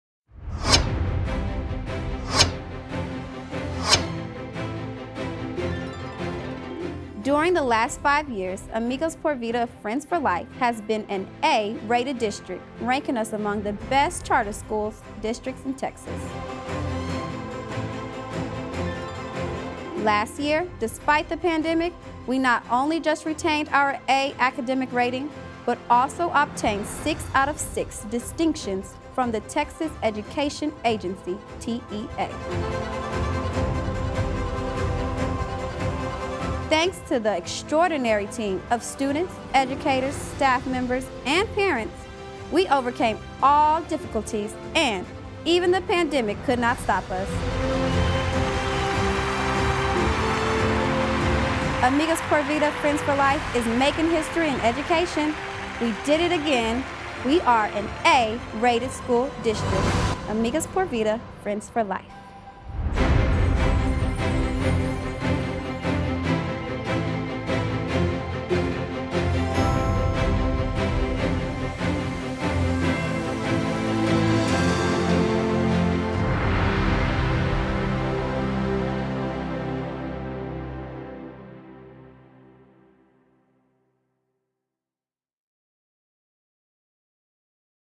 We did it again!! APV-FFL is an "A" rated campus!!
Our school has been among the highest-ranked school districts for the last six years and we did it again. We are an "A" rated school district by the Texas Department of Education - TEA

Amigos por Vida - Friends for Life
Elementary and Middle School